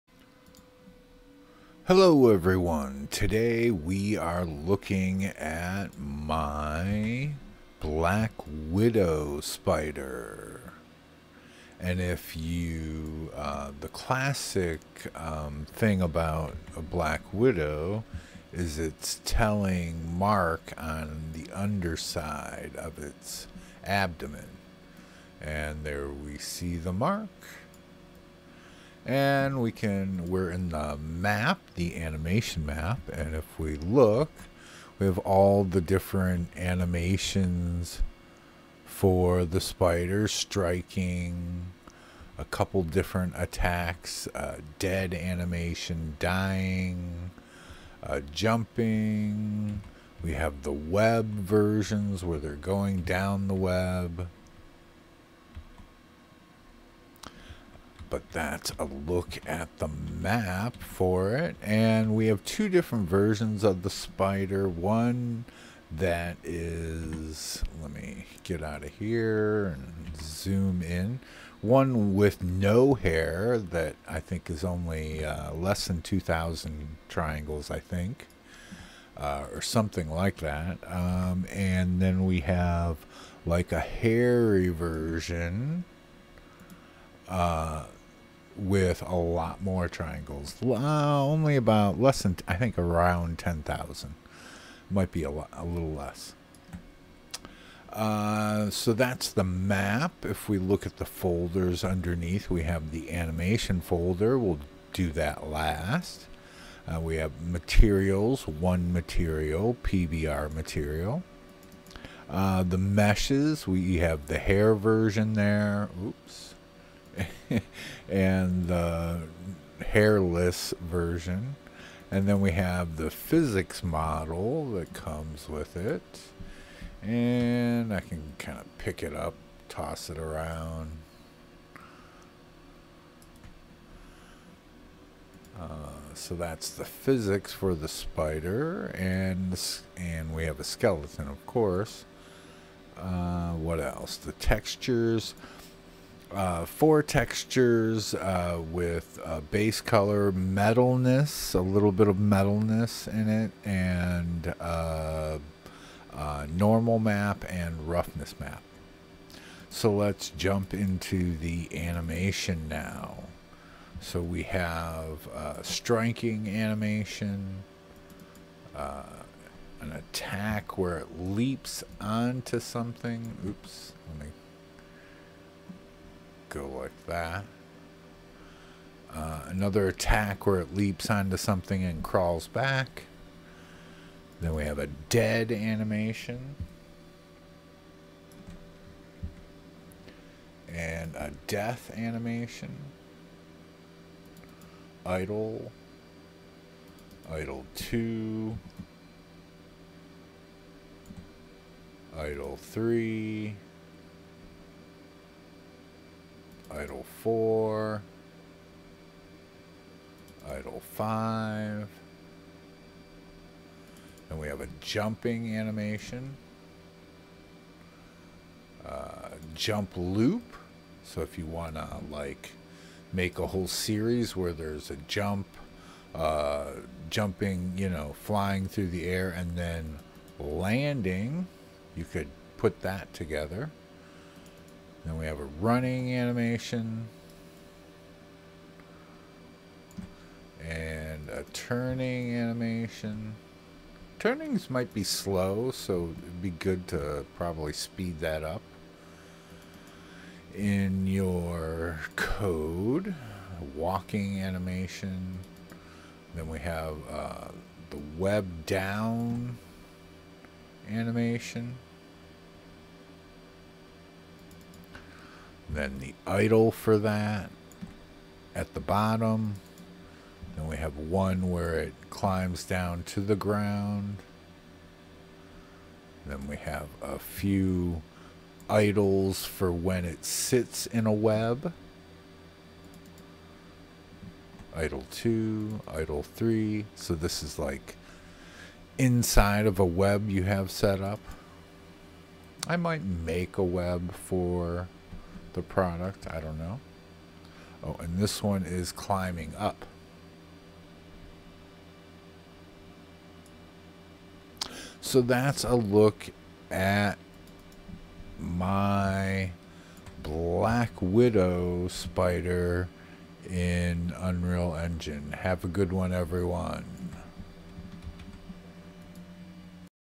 Medhue Black Widow Spider in Unreal Engine!!
This is a quick video to show off my spider that is available on the Unreal Engine Marketplace.